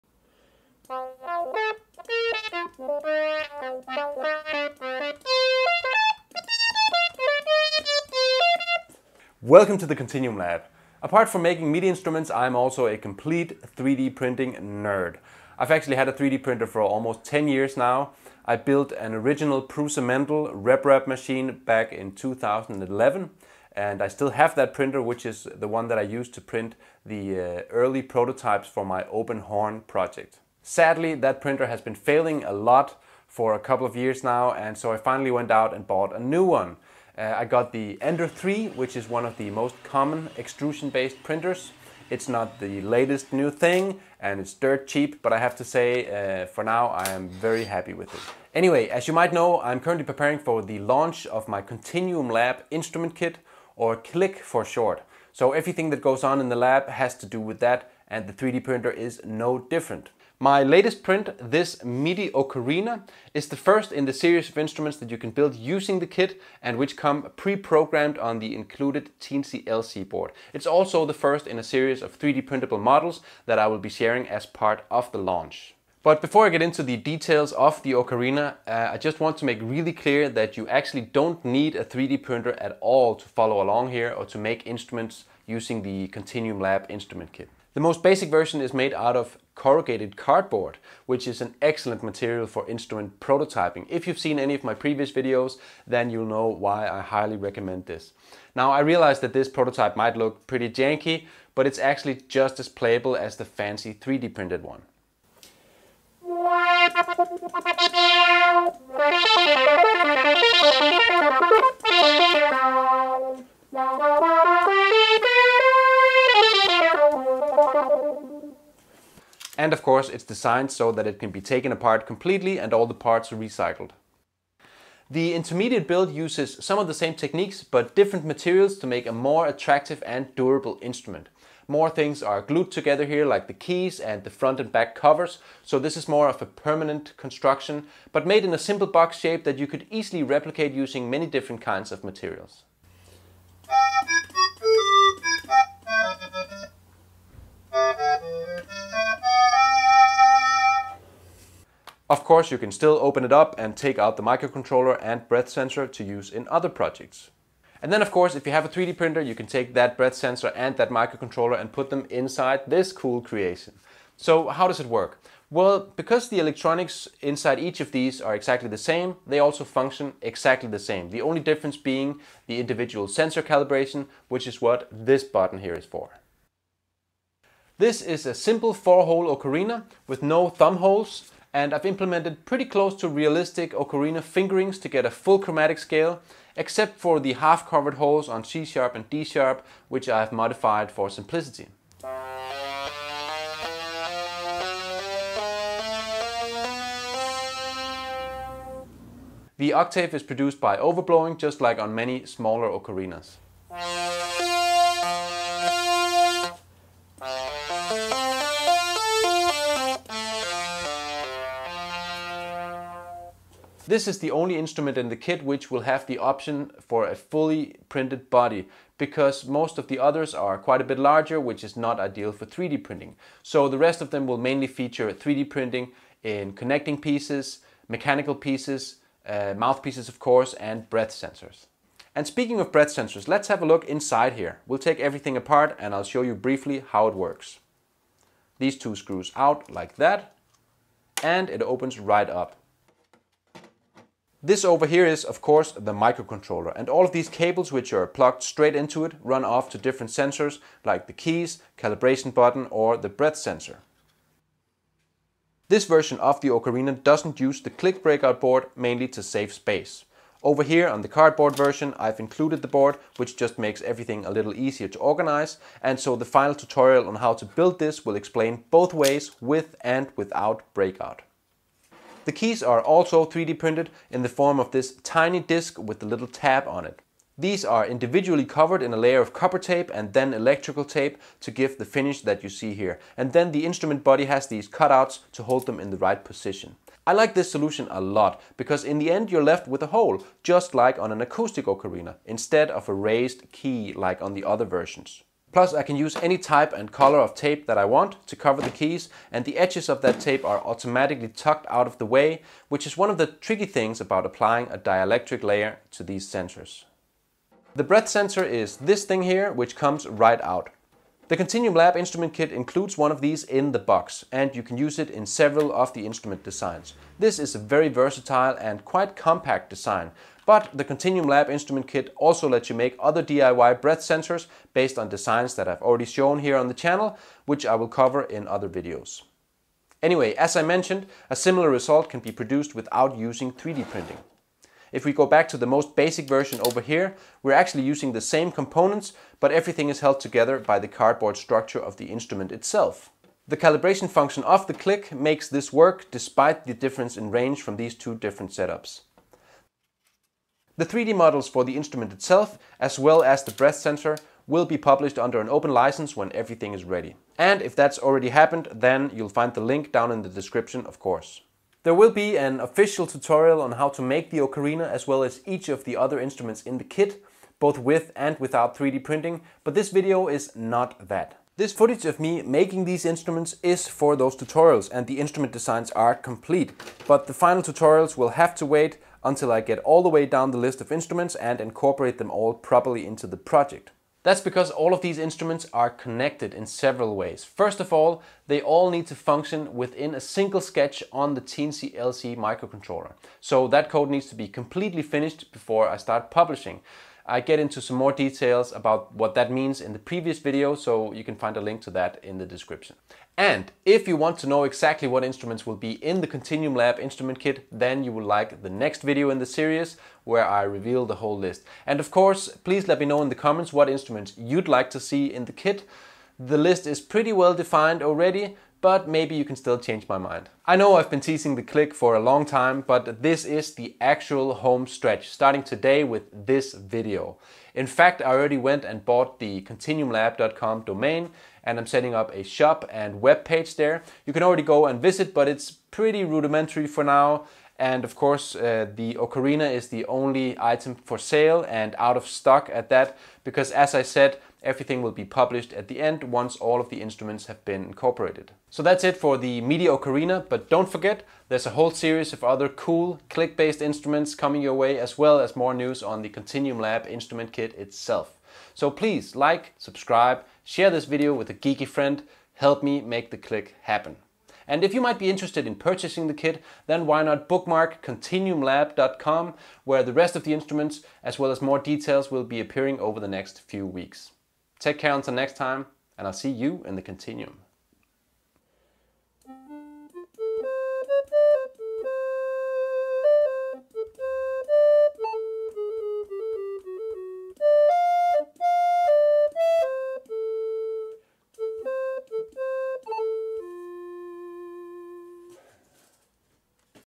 3d-printed MIDI ocarina, and the K.L.I.K. prototypes that got me there.
This ocarina was made using the KontinuumLAB Instrument Kit, which is a DIY kit for making MIDI instruments. The Ocarina went through 3 different iterations to get to the current 3d-printable prototype, and in this video I introduce all of them.

The FULL TUTORIAL and build video on how to make this instrument is now up on the channel.

See you in the Kontinuum!